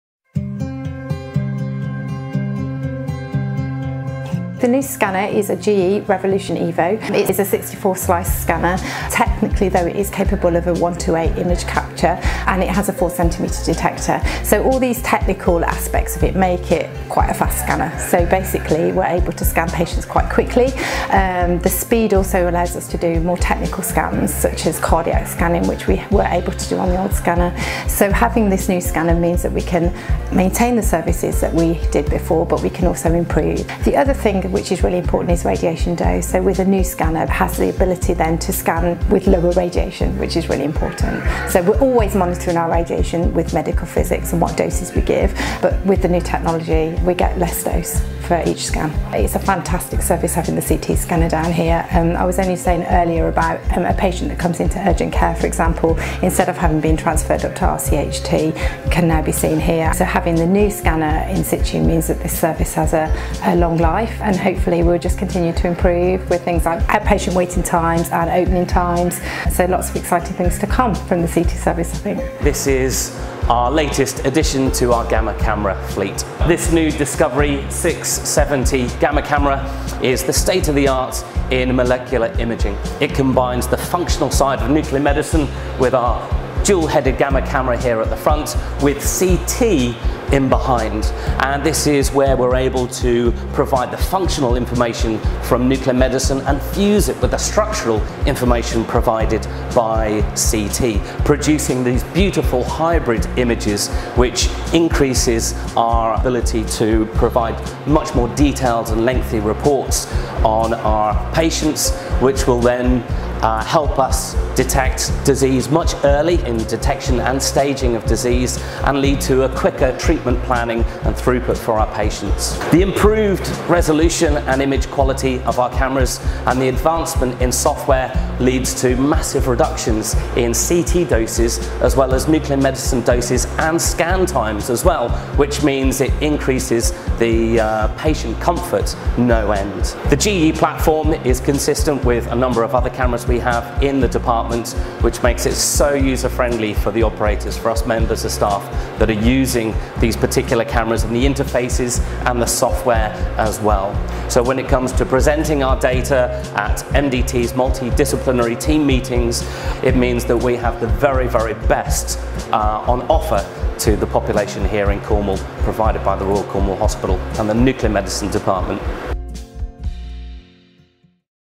New West Cornwall Hospital CT Scanner and Royal Cornwall Hospital Gamma Camera
West Cornwall Hospital and Royal Cornwall Hospital unveil their new CT GE Revelotion Evo Scanner and Discovery 670 Gamma Camera